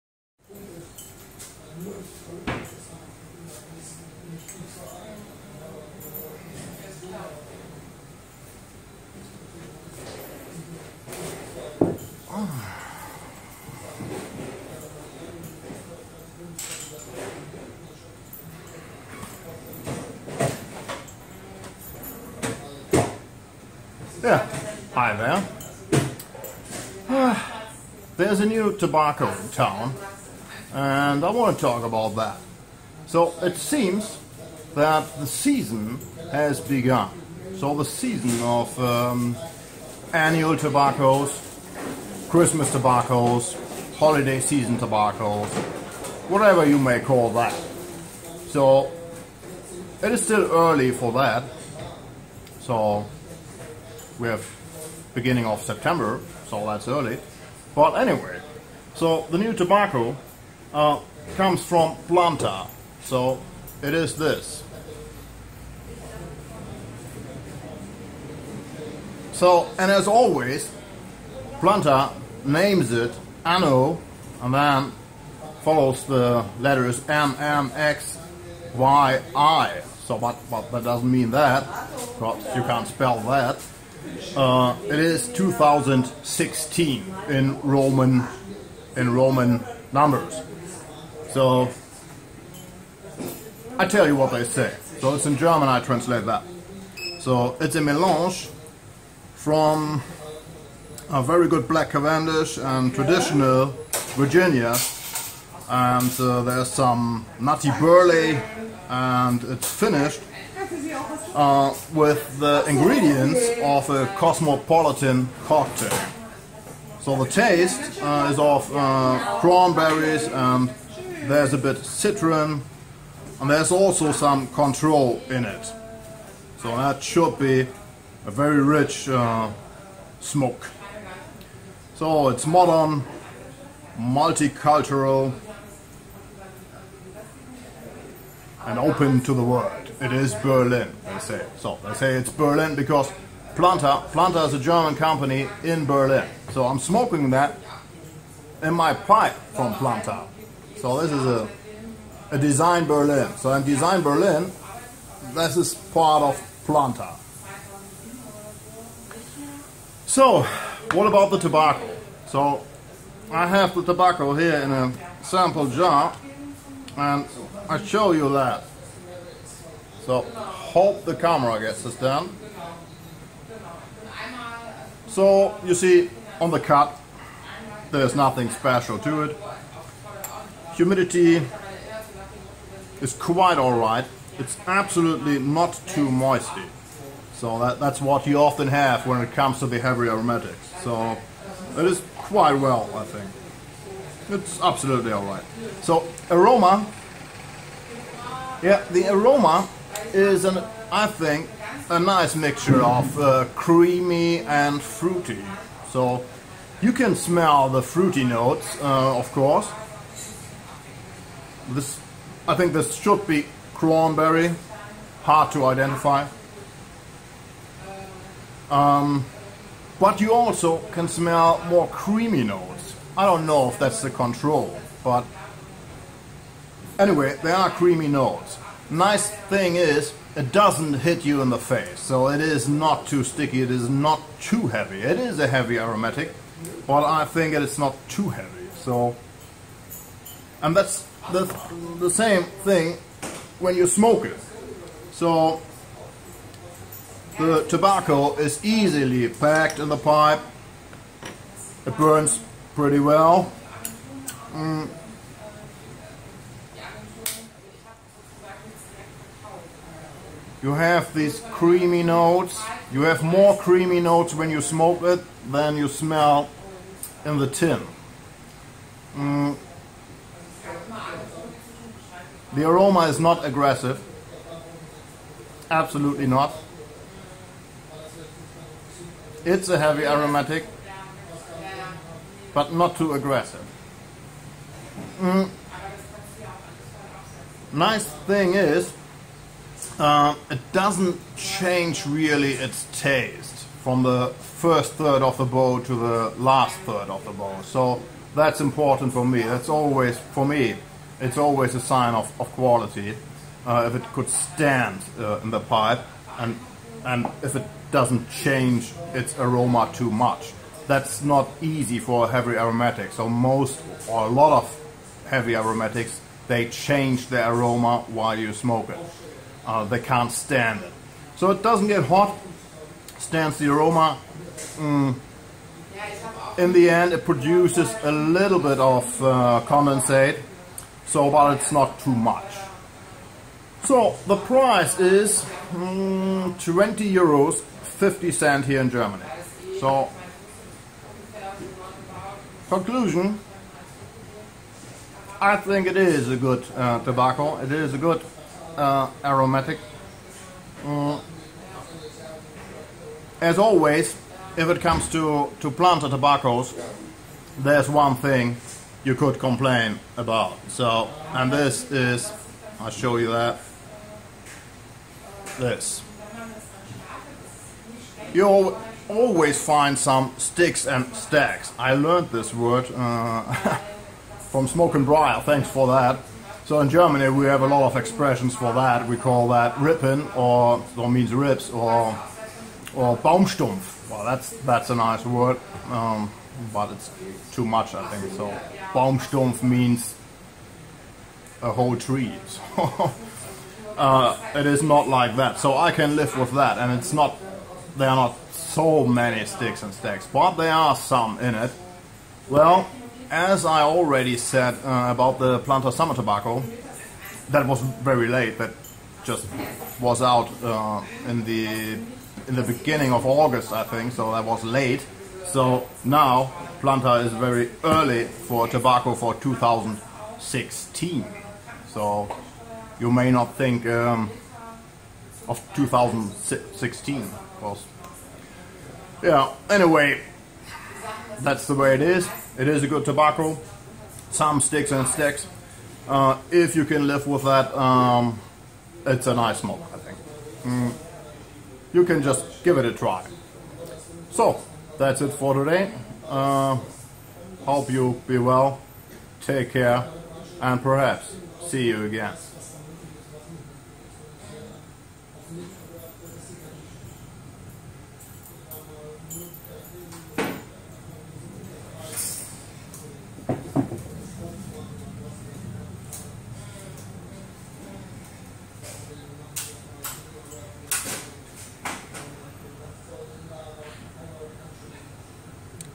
Planta ANNO MMXVI at Ronny`s place
...a short review about Planta`s new tobacco for the year 2016 at the Roland of Bremen tobacco shop...